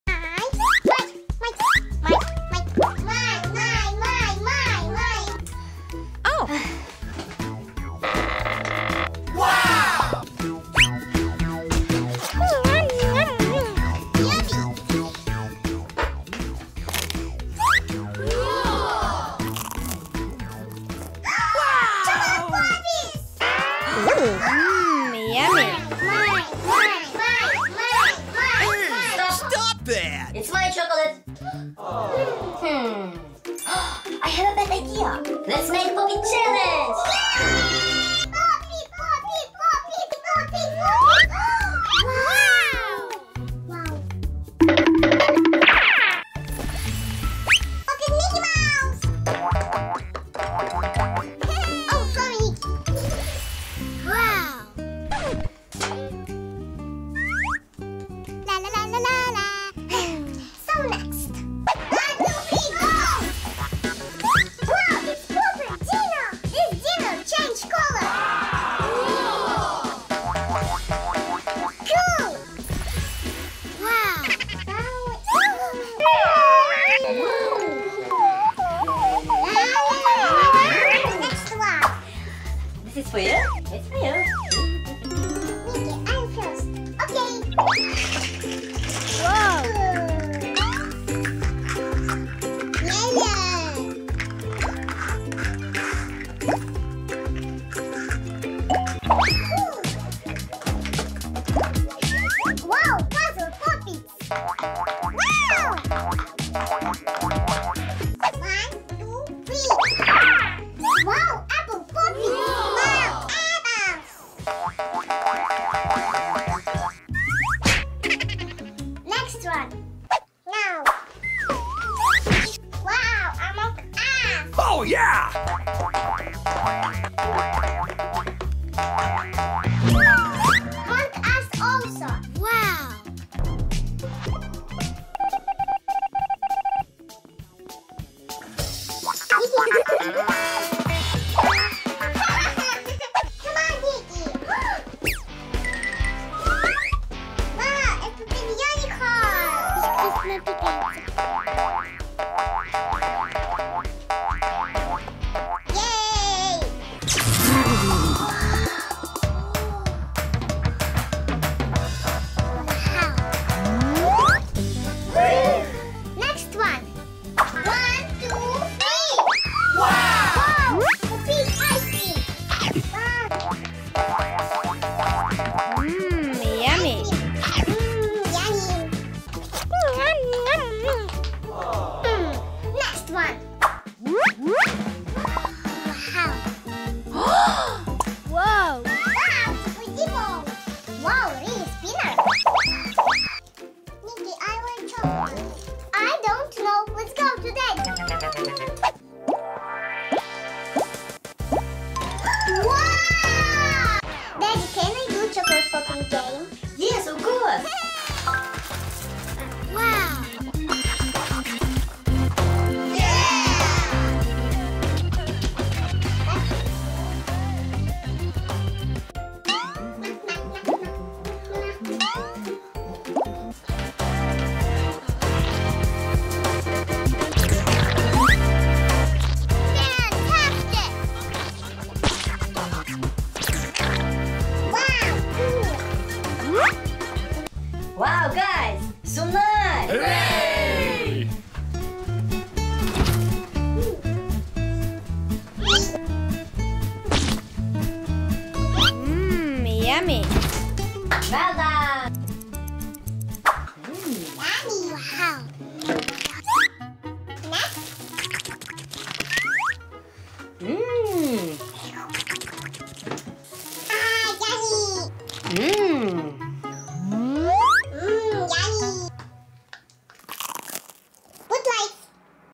Makko and Niki play Pop It Challenge
Makko and Niki love to play Pop It. Dad came up with an interesting game for children. Makko and Niki play pop it challenge.